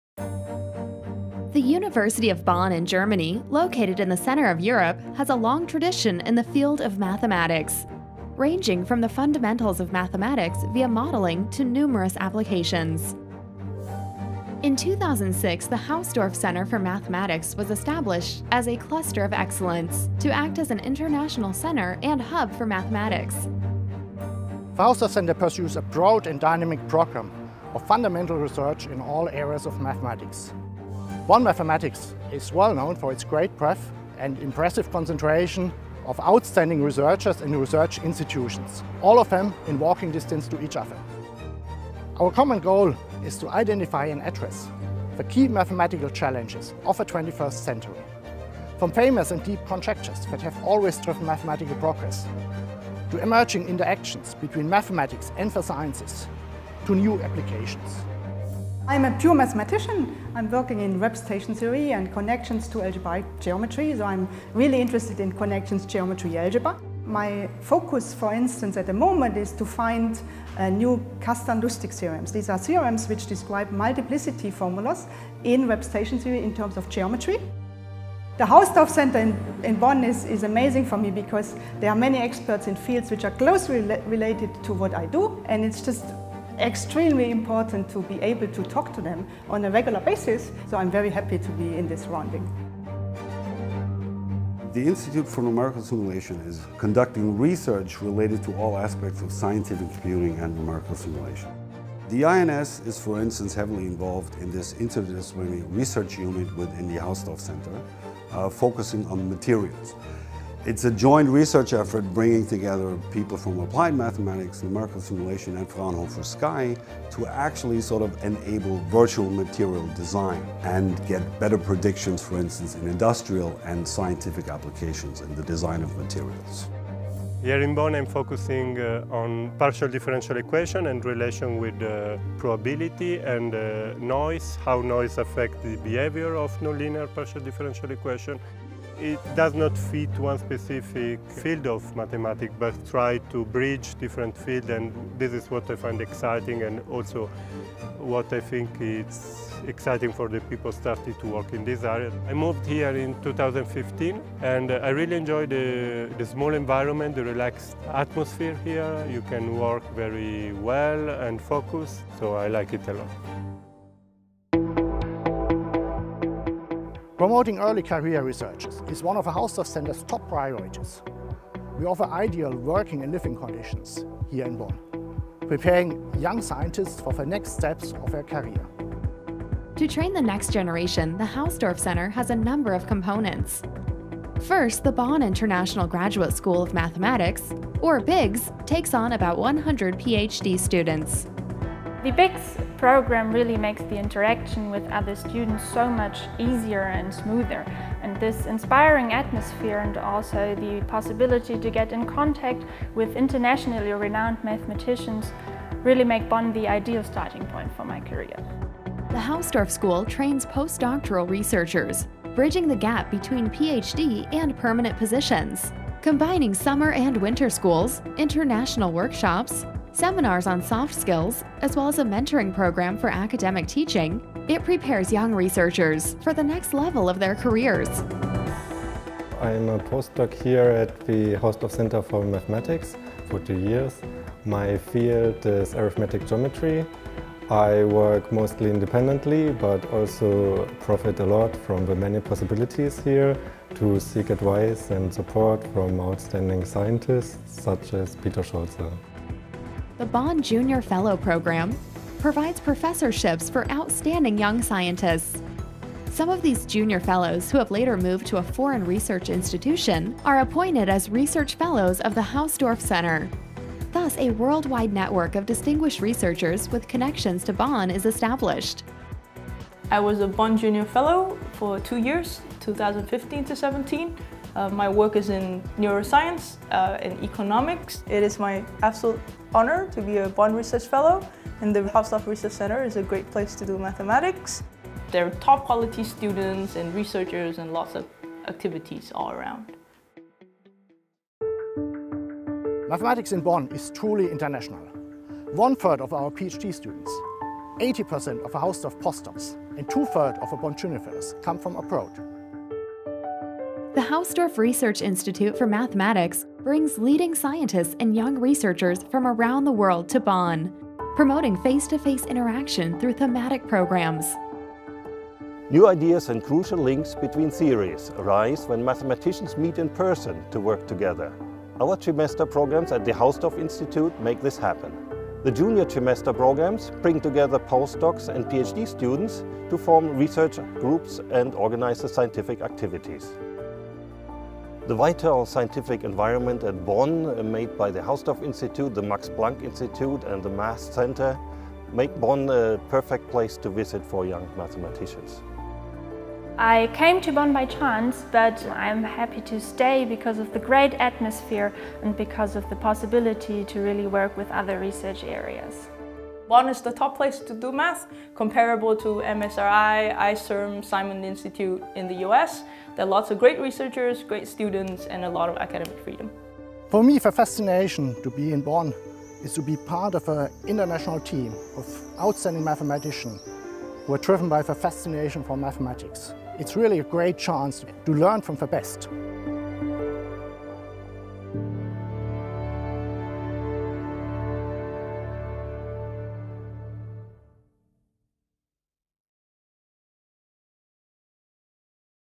Hausdorff Center for Mathematics, Bonn
An International Hub for Mathematics in the Center of Europe

The University of Bonn in Germany has a long tradition in the field of mathematics, home for decades to many world-renowned mathematicians, includign Felix Hausdorff. In 2006, the Hausdorff Center for Mathematics was established as Germany's first and only ‘Cluster of Excellence’ in Mathematics - to act as an international center and hub for experts in the field.

The Hausdorff Center for Mathematics (HCM) capitalizes on a broad vision of mathematics, ranging from pure mathematics, to contributions to quantative modeling in economics and the natural sciences, to industrial applications. HCM strives to serve the international mathematical community as a platform for exchange and research.

Promotion of early-career researchers is also a top priority of the Hausdorff Center, including three main components: the Bonn International Graduate School of Mathematics (BIGS), the Hausdorff School for postdocs, and the Bonn Junior Fellow (BJF) program.

Bonn's history, its support for young researchers, and it's collaborative atmosphere make it one of the best places in the world to do matheamtics.